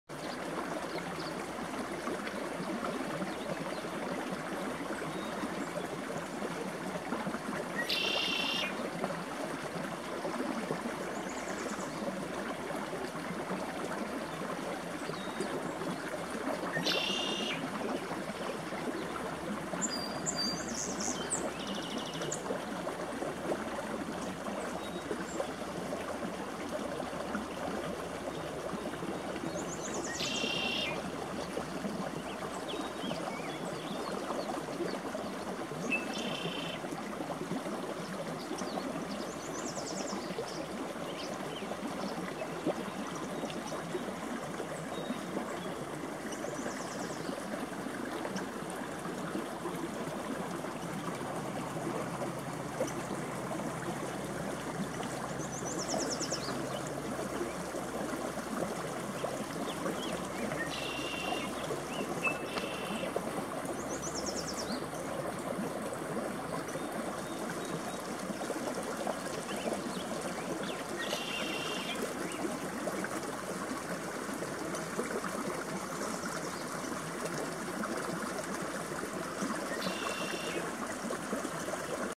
nature sounds- creek and marsh
Beautiful bird sounds along the creek where it runs into the marshy pond.
The best place to be early in the morning.
Thank you to my followers, I love you guys!

I will try to identify these birds for those of you who want to know what they are, I will be more specific when I can.